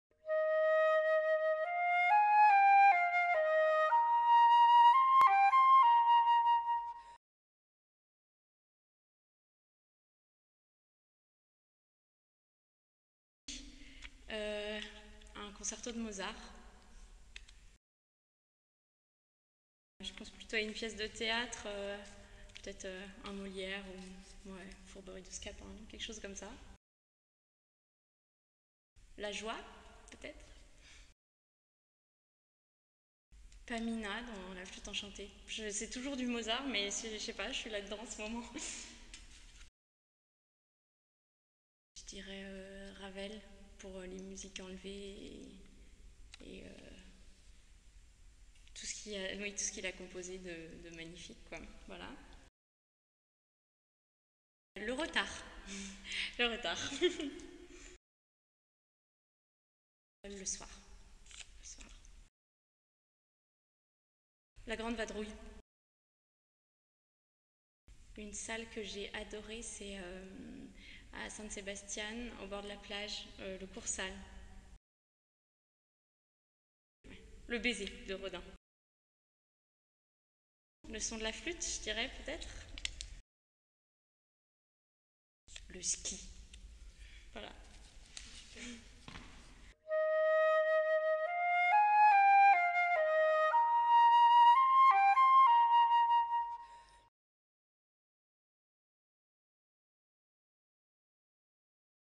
Interview Portrait Chinois - Mathilde Caldérini - Festival Européen Jeunes Talents
Deuxième vidéo de notre série portrait chinois.
A l'occasion du Festival Européen Jeunes Talents 2019, nous avons créé une interview spéciale pour les artistes qui donneront un concert "Carte Blanche".
Réalisation : Pauline Grousset
Conception : Claire de Prekel